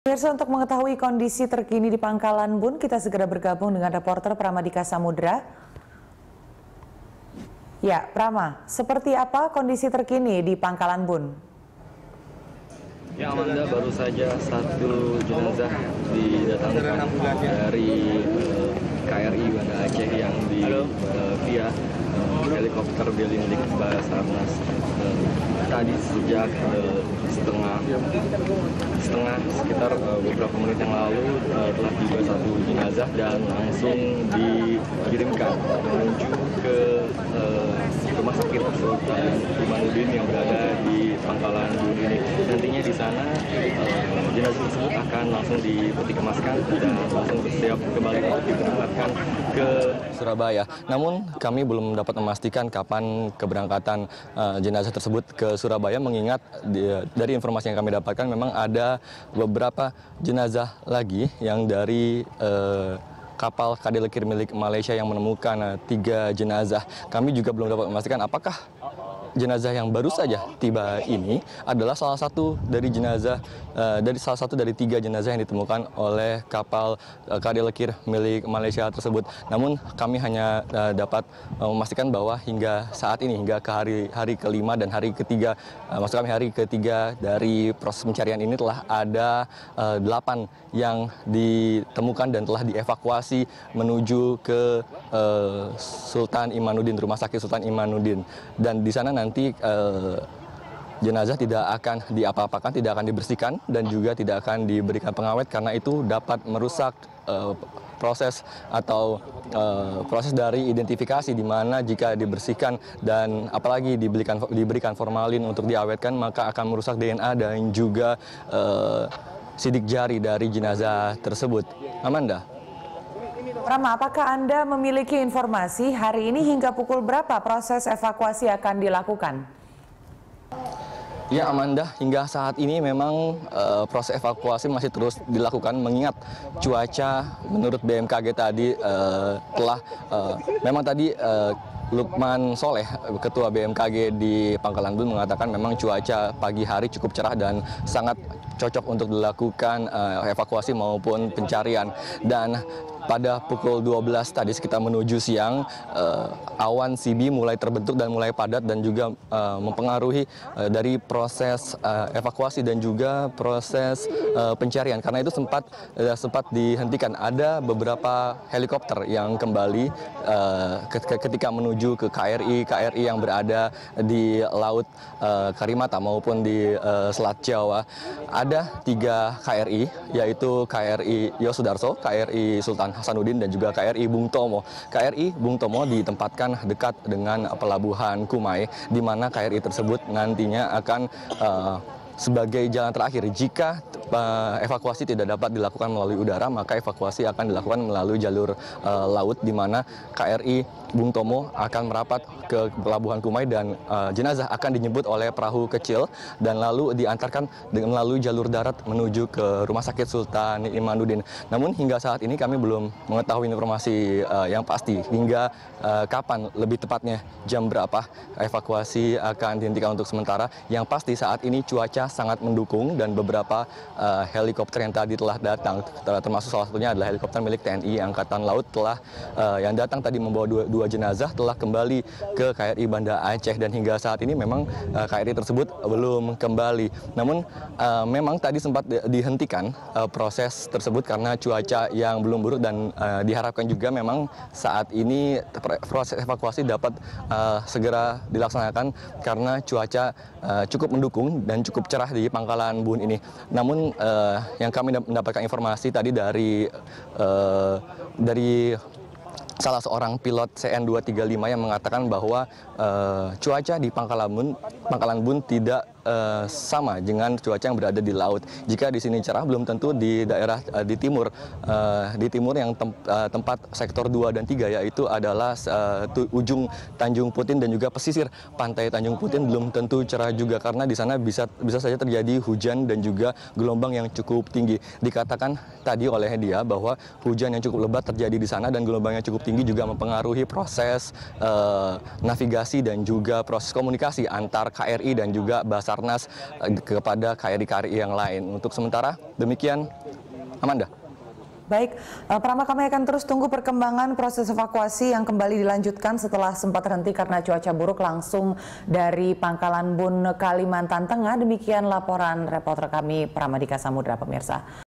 Satu Jenazah Lagi Tiba di Pangkalan Bun
Masih ada satu jenazah penumpang pesawat AirAsia QZ8501 di RSUD Sultan Imanudin, Pangkalan Bun, Kalimantan Tengah.

Facebook.com/BeritaSatuTV
Youtube.com/BeritaSatu